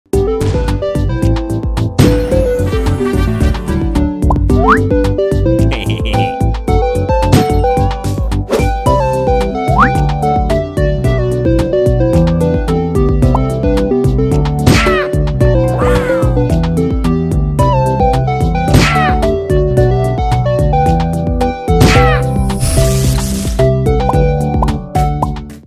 231215 EN HSA 1Play mov25s 16x9 jh1 sy
Who will become "it?"
Camouflage as one of the randomly spawned objects depending on the theme, blend in with your surroundings, and outwit whoever's "it" in this game of stealth!

[Features]

- Play online with up to 15 players
- Large-scale asymmetric matches with 5 players VS 10 players
- 19 stages with 4 different themes
- Transform into different objects and hide in unlikely places to outwit your opponents!
- Use special skills that are randomly generated on the stage to quickly find hidden objects!